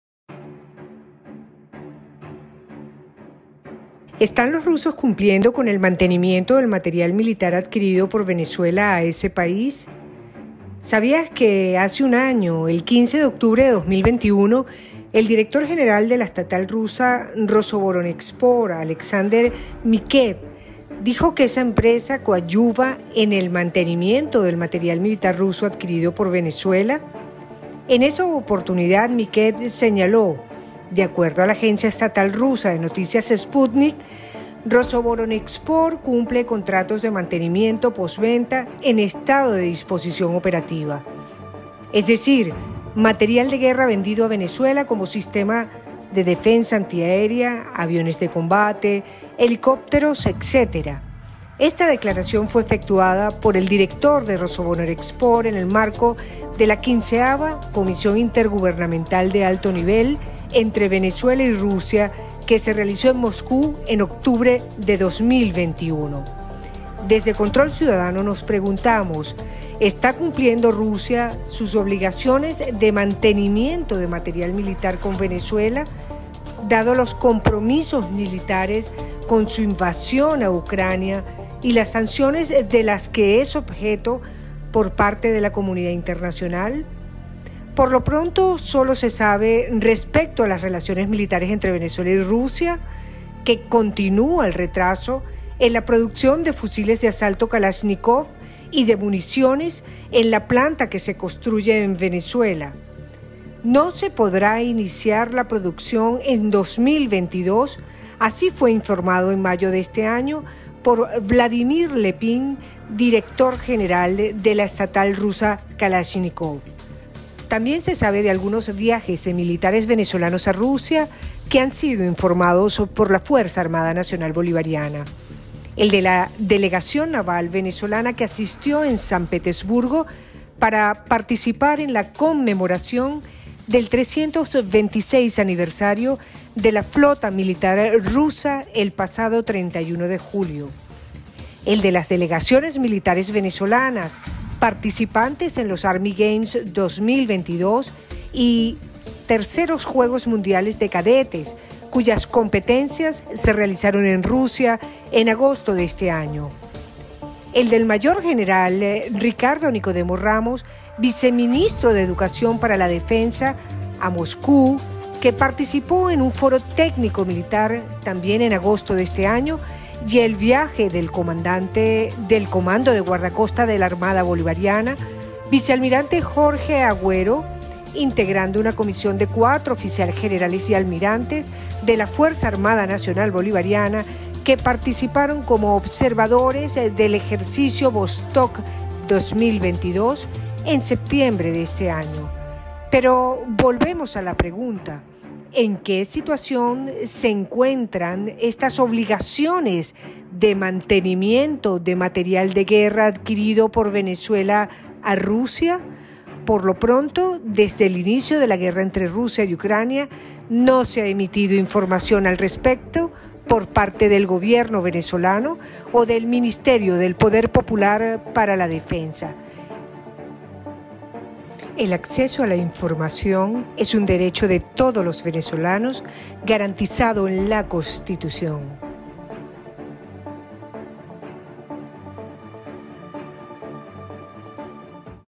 Está cumpliendo Rusia sus obligaciones de mantenimiento de material militar adquirido por Venezuela?
Desde el inicio de la guerra entre Rusia y Ucrania, no se ha emitido información al respecto.

El acceso a la información es un derecho de todos los venezolanos, garantizado en la Constitución

Producción y edición: Miranda Díaz